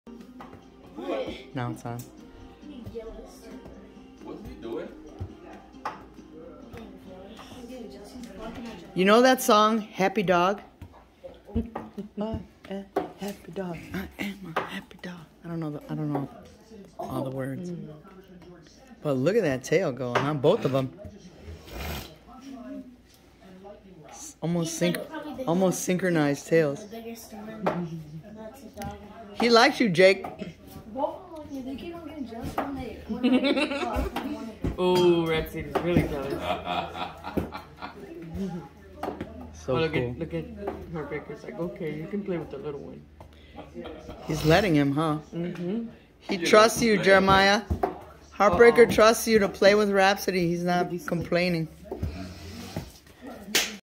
Great Dane Love Heartbreaker, Jacob and Jeremiah
Grandsons Jacob & Jeremiah with Rhapsody and The Heartbreaker